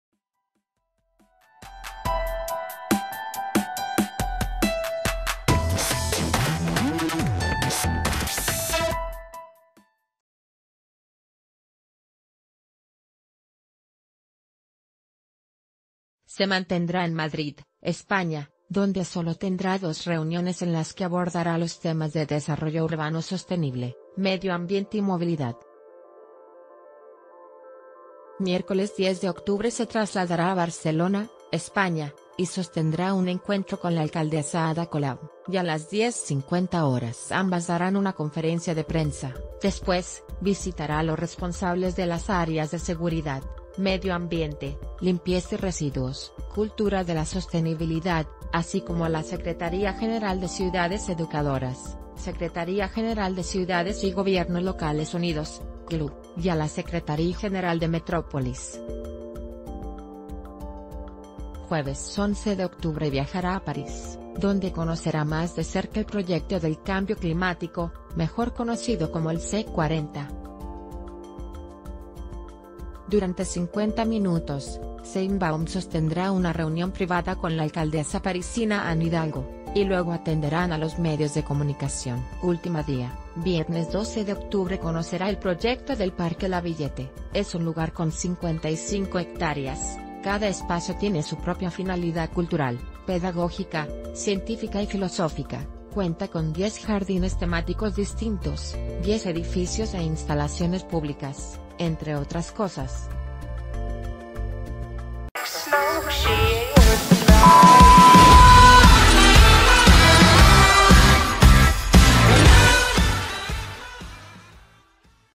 Así transcurre la gira por Europa de la jefa de gobierno electa Claudia Sheinbaum
Se mantendrá en Madrid, España, donde sólo tendrá dos reuniones en las que abordará los temas de Desarrollo Urbano Sostenible, Medio Ambiente y Movilidad. Miércoles 10 de octubre Se trasladará a Barcelona, España, y sostendrá un encuentro con la alcaldesa Ada Colau, y a las 10:50 horas ambas darán u...